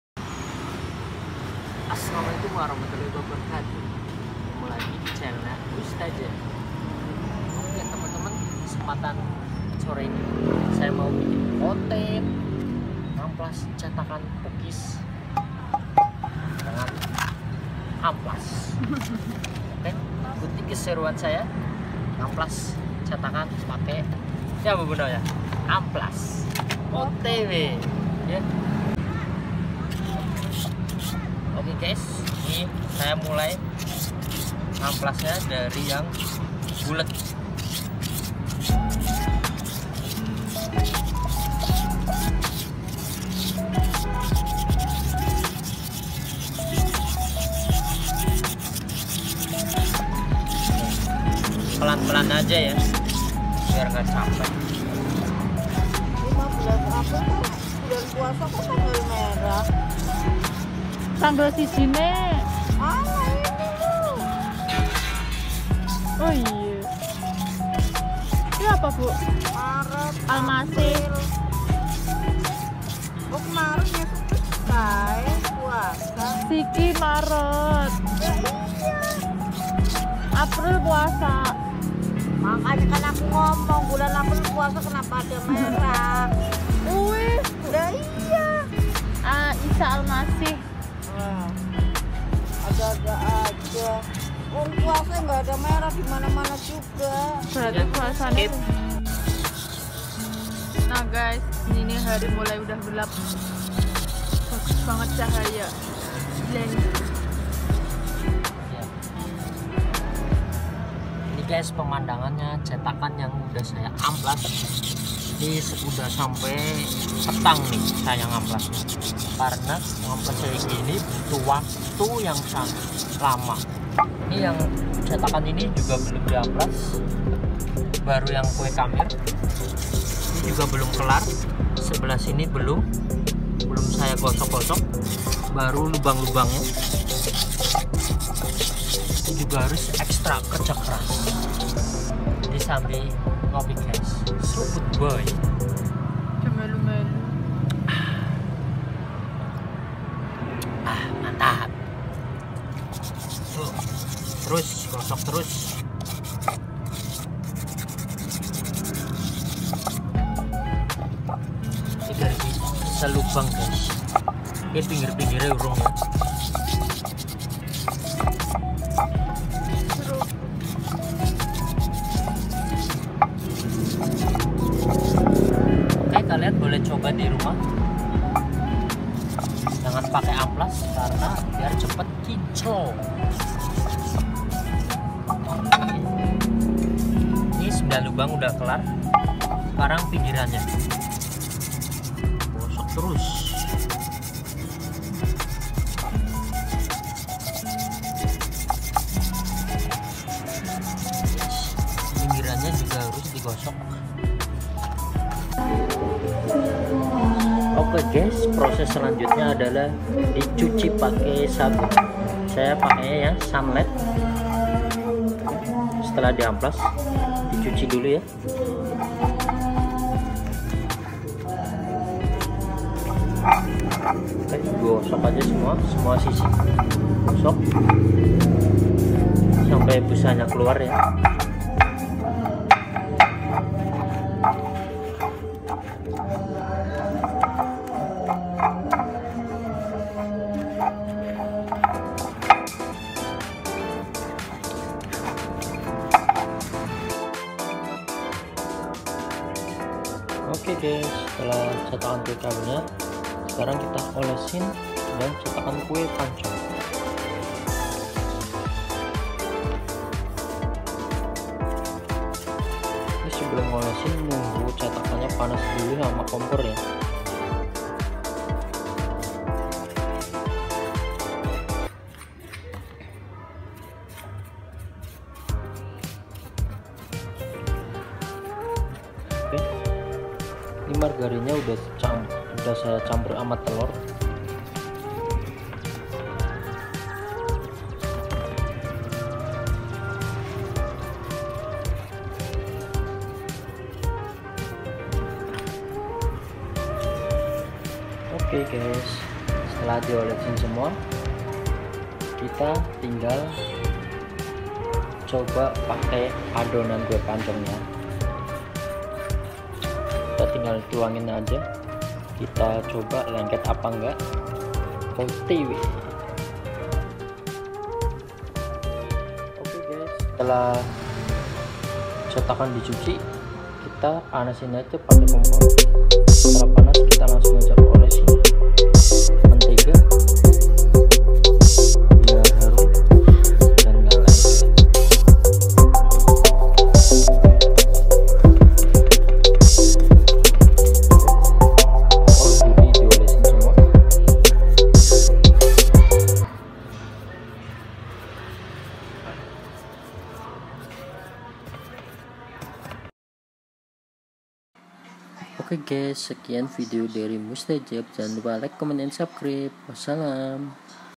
Cara agar cetakan pukis & khamir seperti baru dan tidak lengket#pukis#cetakanpukis
hallo guys.kli ini saya mau bagi tips agar cetakan kue pukis dan khmir tidak lengket dan seperti baru.
bahan2
amplas kasar/halus bisa
telor ayam
margarin.